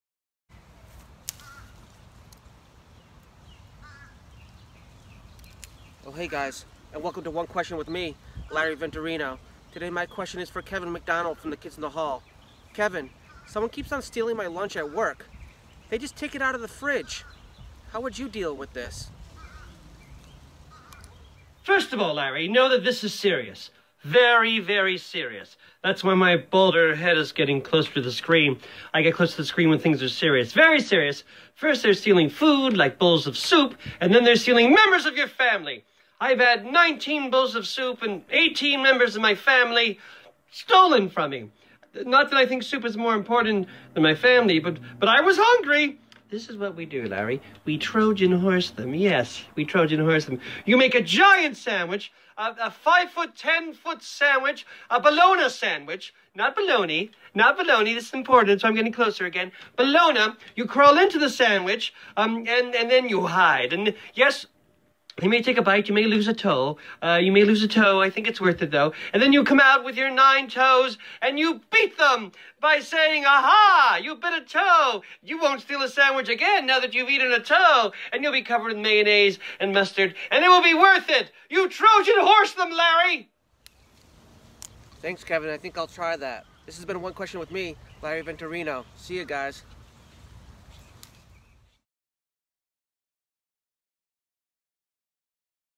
Kevin McDonald & Larry Venturino
One Question With Larry Venturino - #28 Kevin McDonald from the Kids In The Hall.

Stand Up Comedian, Larry Venturino gets advice from comedy legend, Kevin McDonald, from the Kids In The Hall.